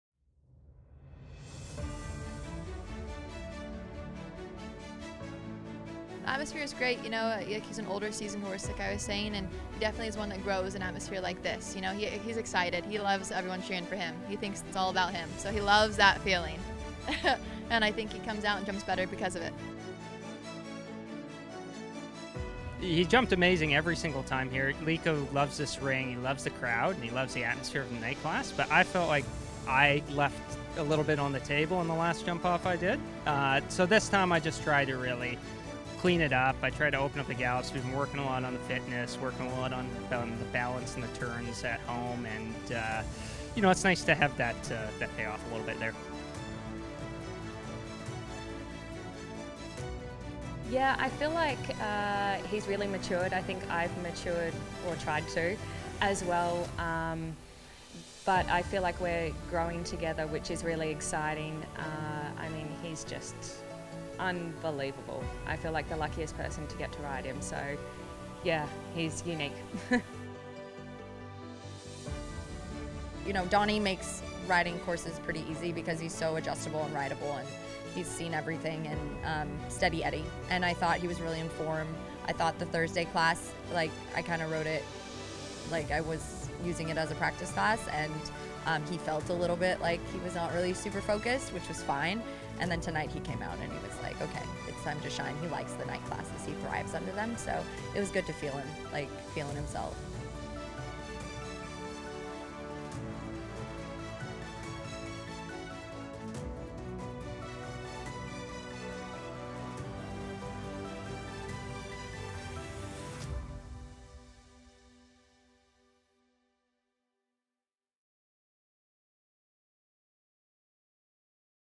2023 Ocala Summer Series Fédération Equestre International (FEI) CSI3* Recap
The first four weeks of the 2023 Ocala Summer Series welcomed some of the best international show jumping athletes to compete in FEI CSI3* competition at World Equestrian Center – Ocala. Exhibitors and spectators enjoyed thrilling international competition daily, while the Saturday night grands prix became the featured spectacle. The WEC Grand Arena was brimming with excitement each Saturday night as stunning equine athletes negotiated massive jumps and the crowds roared with approval while winners took their rightful spots atop the podium each week.